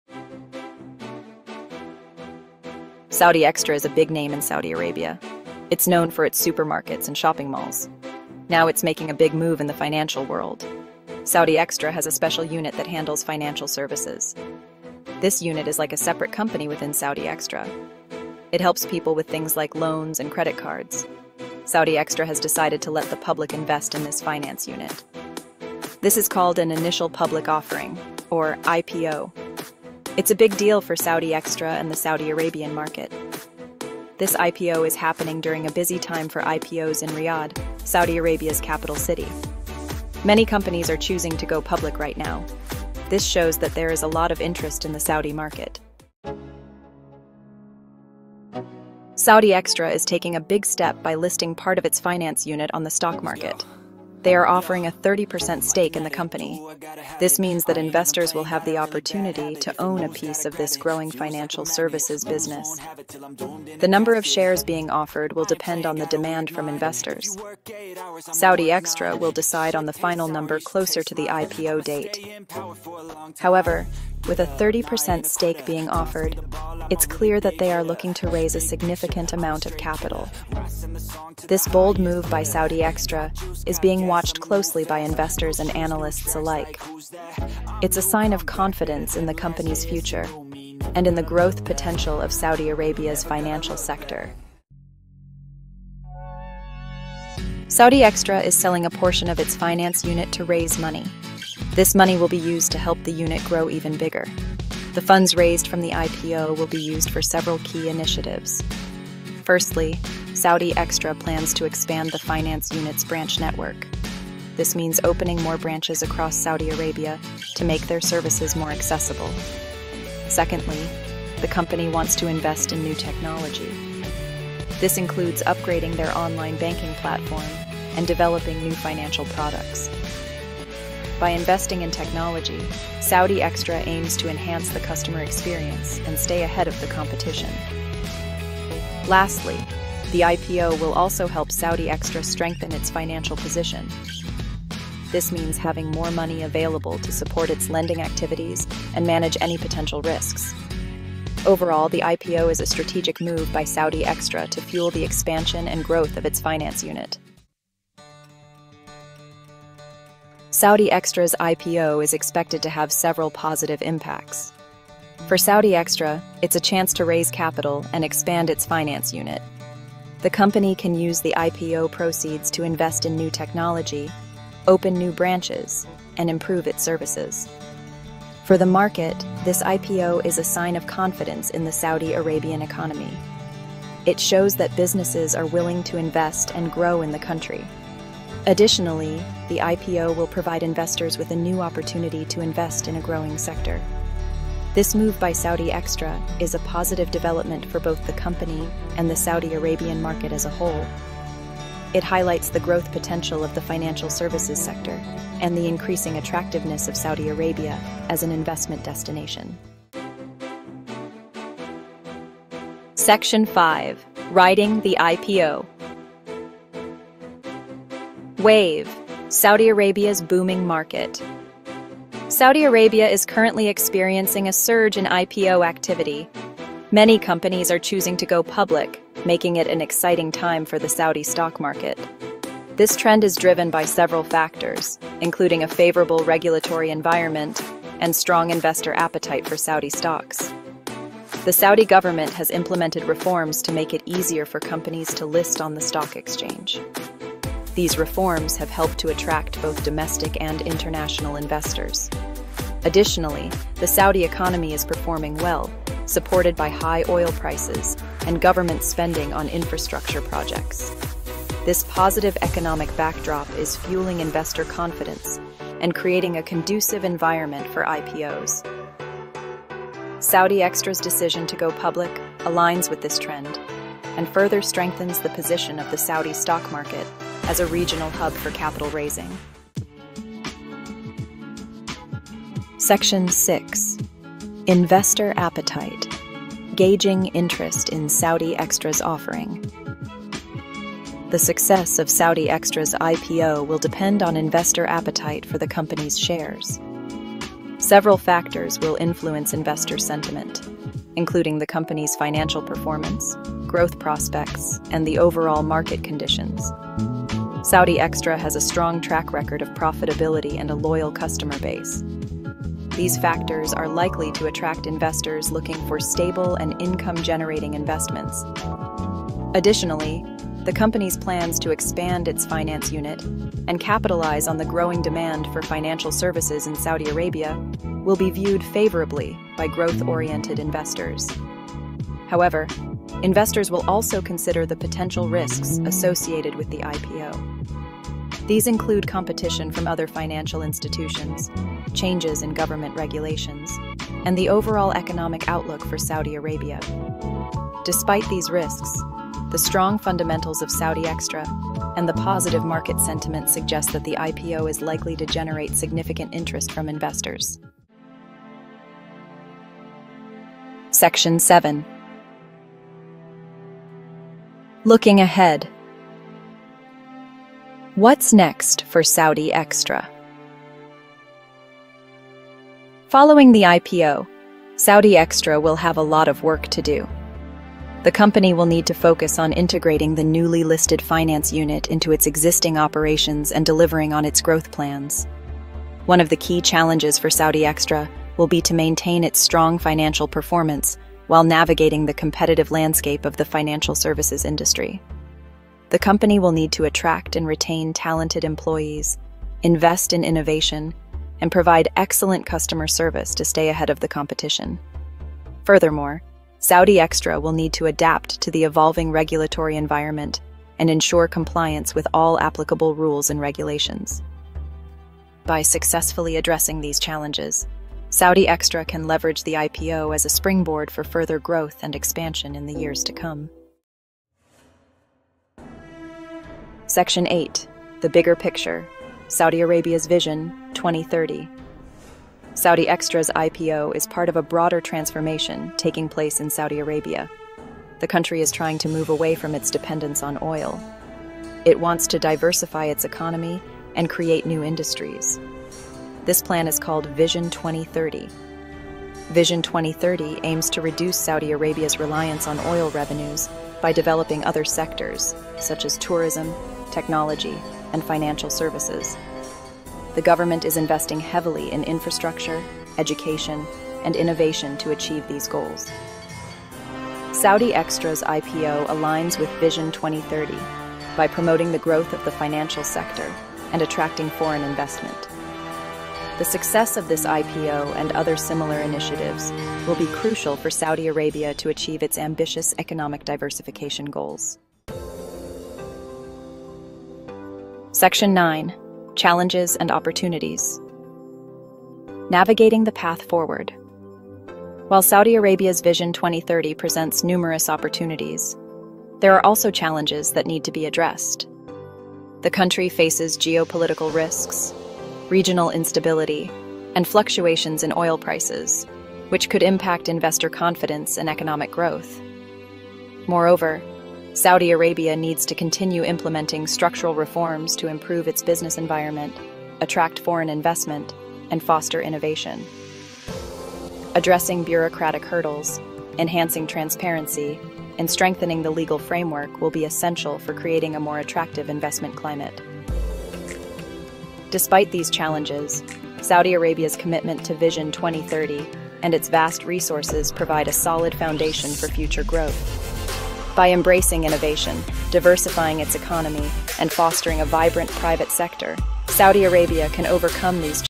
Saudi Extra: 30% Stake IPO - A Game Changer in Finance!
OUTLINE:
00:00:00 Introducing Saudi Extra
00:00:46 Saudi Extra's Finance Unit Goes Public
00:01:34 The Purpose Behind the IPO
00:02:37 Benefits for Saudi Extra and the Market
00:03:29 Saudi Arabia's Booming Market
00:04:39 Gauging Interest in Saudi Extra's Offering
00:05:57 What's Next for Saudi Extra?
00:07:03 Saudi Arabia's Vision 2030
00:08:04 Navigating the Path Forward
00:09:14 A Transformative Step for Saudi Extra

In today’s video, we dive deep into the exciting news about Saudi Arabia's United Electronics Co. (Extra) as they announce a groundbreaking **30% stake IPO** of their financial services unit! 📈✨

📍 **Key Highlights:**
- **Location:** Riyadh, Saudi Arabia 🕌
- **Company:** United Electronics Co. (Extra) 🏢
- **Stake Offered:** 30% of the financial services unit 💰
- **Shares to be Floated:** 7.5 million shares 📊
- **Parent Company:** United International Holding Co. 🌍
- **Subsidiary:** Tasheel Finance 💳

🔍 We discuss what this means for investors, the potential impact on the Saudi stock market, and why this IPO is a significant milestone in the finance sector! 💡

💬 **Join the conversation!** What are your thoughts on this IPO? Will you be investing? ⬇️